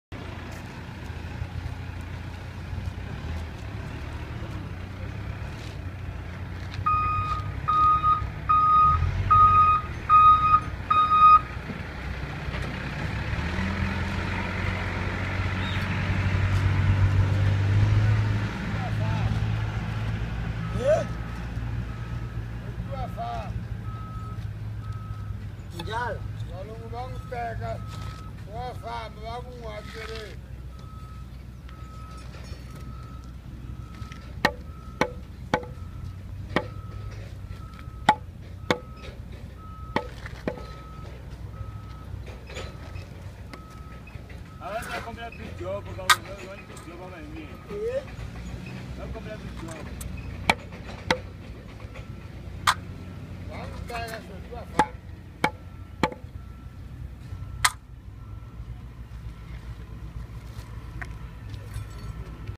Kerb installation the difficult way
Bosun Castle Bottom kerbs installed without the correct tools